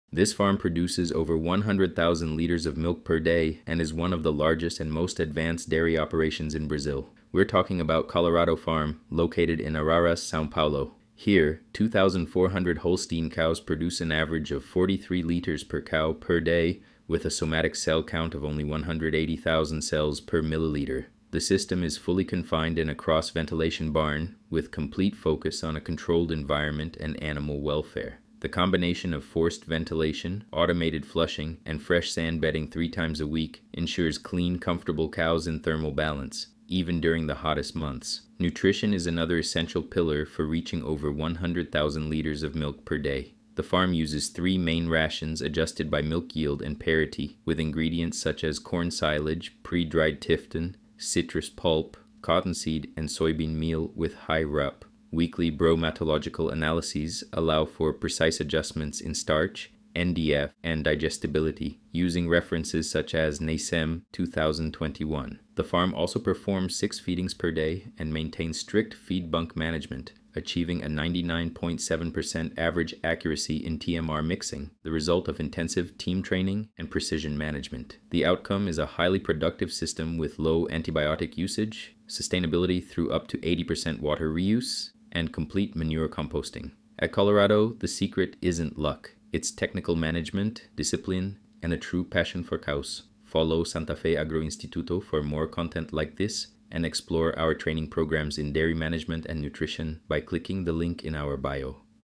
This farm produces over 100,000 liters of milk per day.
Over 100,000 liters of milk per day, 2,400 cows, and a system that combines science, management, and well-being.

Colorado Farm demonstrates that productivity and sustainability go hand in hand when there is technique and consistency in every detail of the operation.

Would you like to receive more content like this? Follow Santafé Agroinstituto.